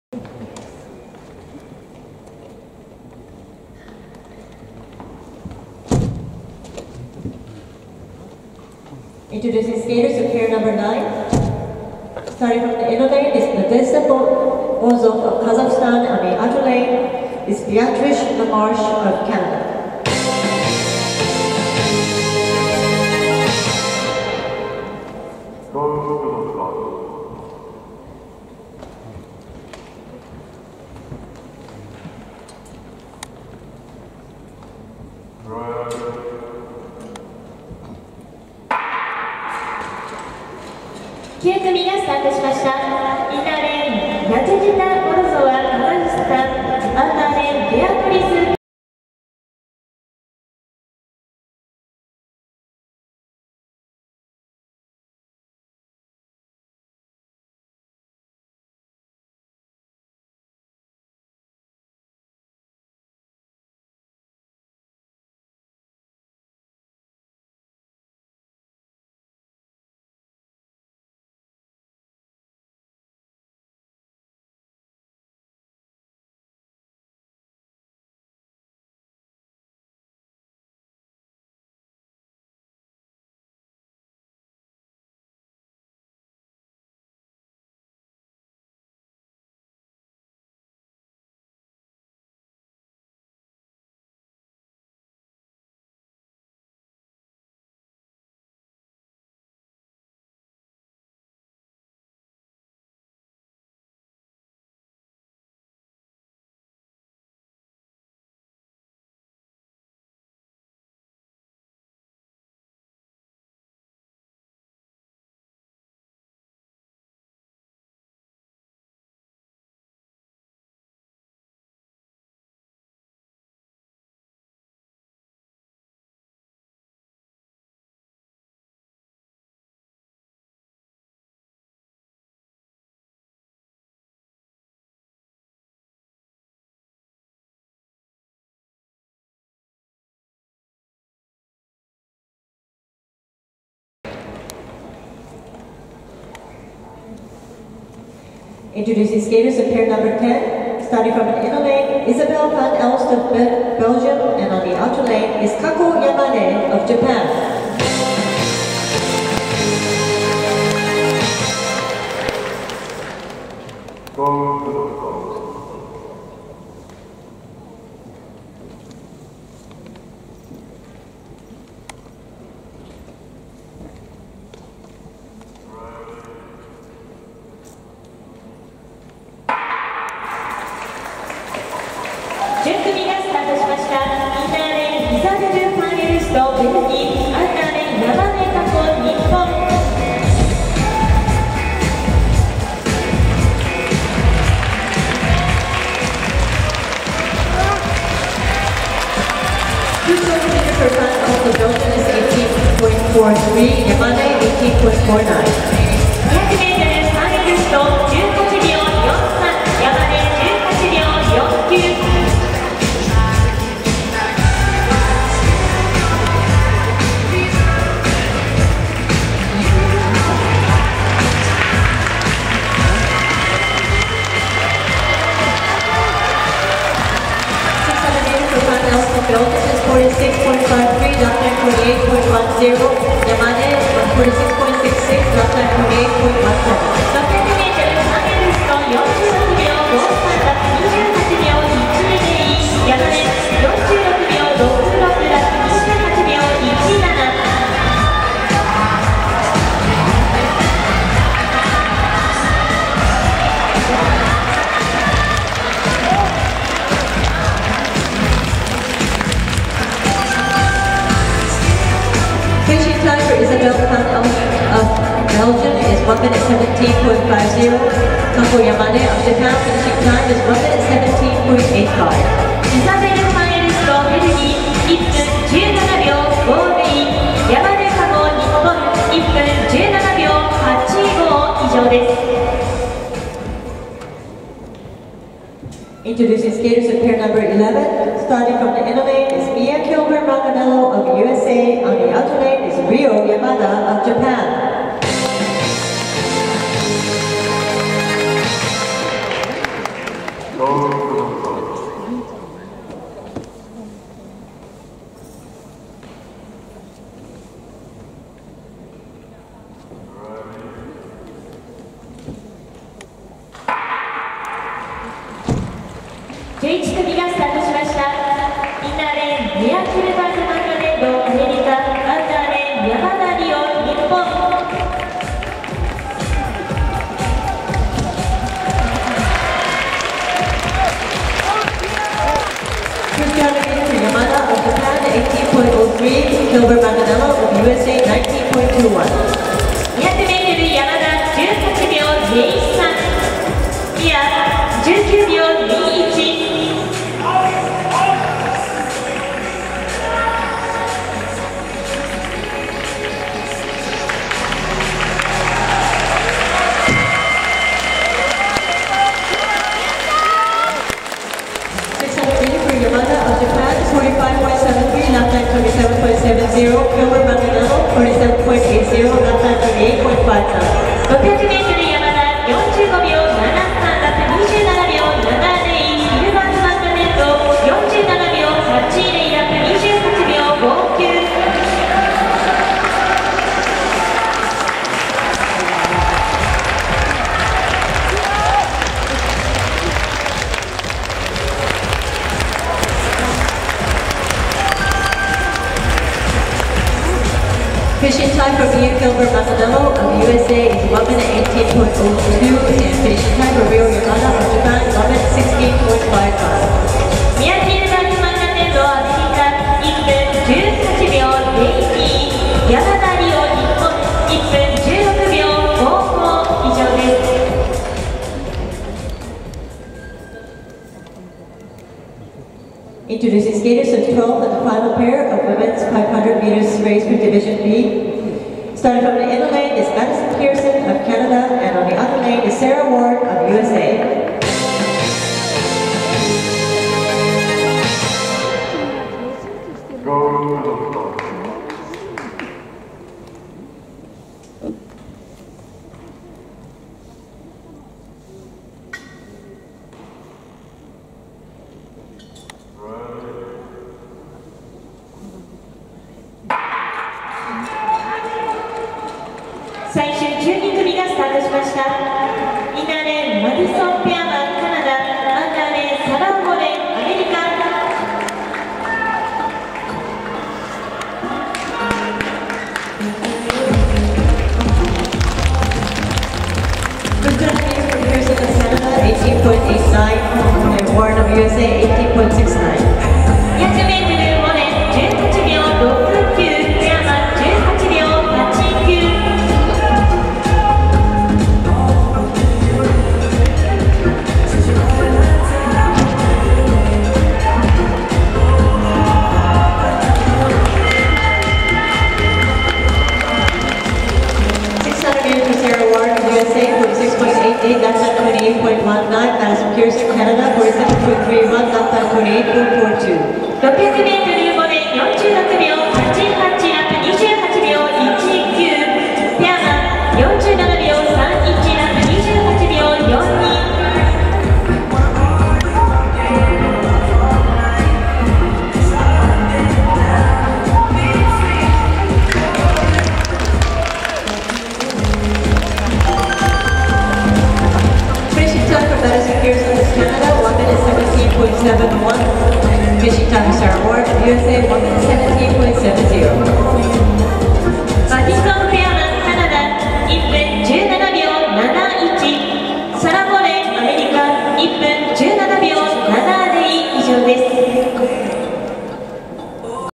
Rio Yamada wins Women 1000m Division B. ISU World Cup Speed Skating 2023/24 Obihiro,Japan.
result:Women 1000m Division B
9pair
IN  Nadezhda Morozova KAZ 1'17"14(3)
OUT  Beatrice Lamarche  CAN 1'17"90(9)
10pair
IN  Isabelle  Van Elst  BEL  1'17"50(4)
OUT    Kako Yamane  JPN  1'17"85(8)
11pair
IN   Mia kilburg-manganello USA 1'18"01(11)
OUT  Rio Yamada  JPN  1'16"55(1)
12pair
IN  Maddison Pearman  CAN 1'17"71(6)
OUT  Sarah Warren  USA  1'17"70(5)

I don’ｔ make a video 2nd place Alina Dauranova.

関連動画：

山根佳子選手が女子500ｍ優勝。2019/2020ジャパンカップスピードスケート競技会第１戦。